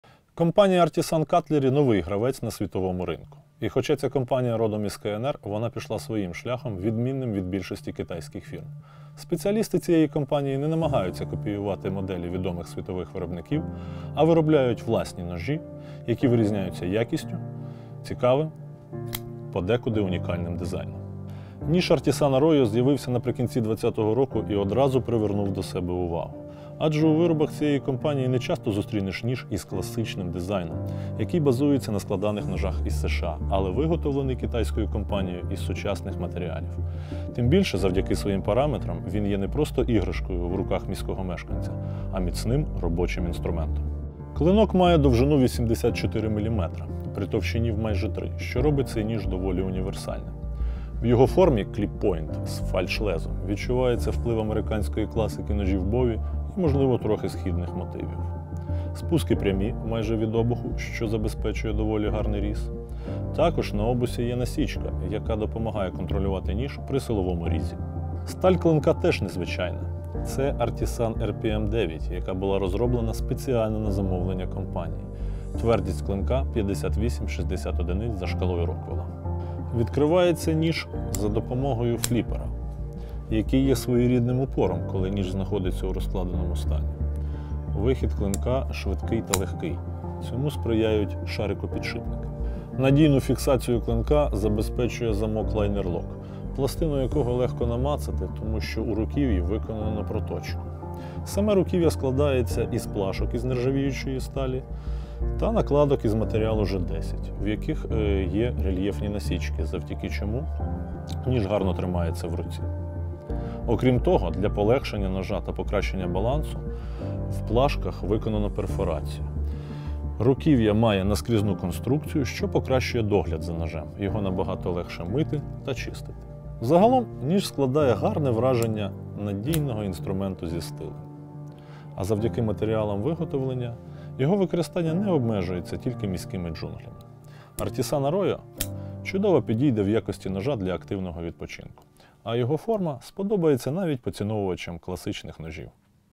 Artisan Arroyo | Ножі та інструменти | Ібіс
У цьому відео ми познайомимо Вас з представником другого покоління складаних ножів від Artisan - модель Arroyo. Цей ніж відрізняється цікавим дизайном, який наділяє його гарними ріжучими властивостями. Але найцікавіше в цьому ножі це сталь, тепер тут не D2, а фірмова порошкова AR-RPM9. Більше - у відео.